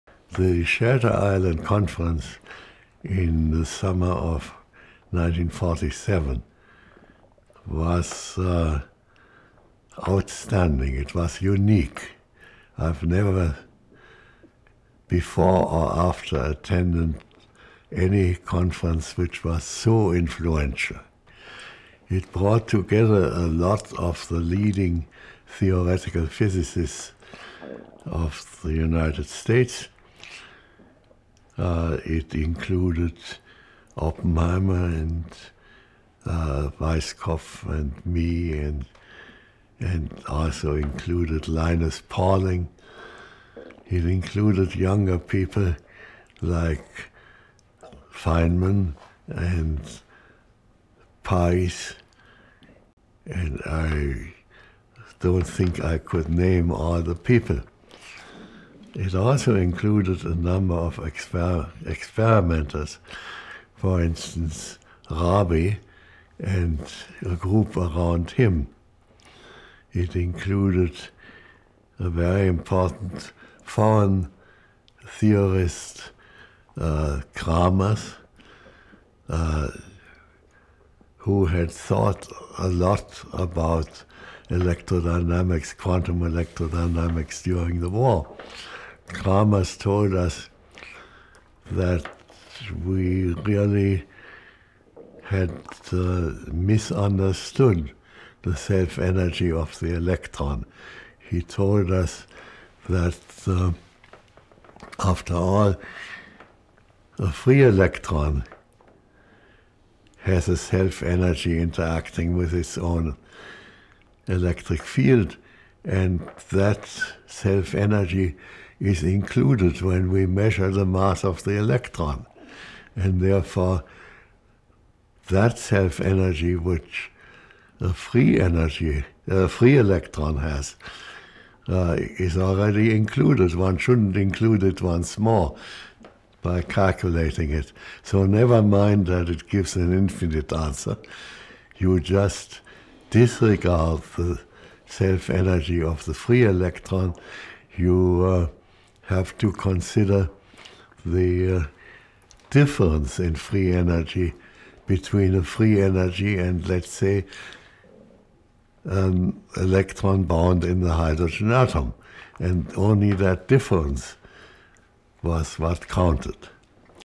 Hans Bethe - The Shelter Island Conference (102/158)
TRANSCRIPT: The Shelter Island Conference in the summer of 1947 was outstanding, it was unique. I have never before or after attended any conference which was so influential. It brought together a lot of the leading theoretical physicists of the United States. It included Oppenheimer and Weisskopf and me, and it also included Linus Pauling. It included younger people like Feynman and Pais. And I don't think I could name all the people. It also included a number of experimenters, for instance, Rabi, and a group around him. It included a very important foreign theorist, Kramers, who had thought a lot about electrodynamics, quantum electrodynamics, during the war. Kramers told us that we really had misunderstood the self energy of the electron. He told us that, after all, the free electron has a self energy interacting with its own electric field, and that self energy is included when we measure the mass of the electron. And therefore, that self energy, which a free electron has, is already included, one shouldn't include it once more by calculating it. So never mind that it gives an infinite answer, you just disregard the self energy of the free electron, you have to consider the difference in free energy between the free energy [electron] and, let's say, an electron bound in the hydrogen atom, and only the difference was what counted.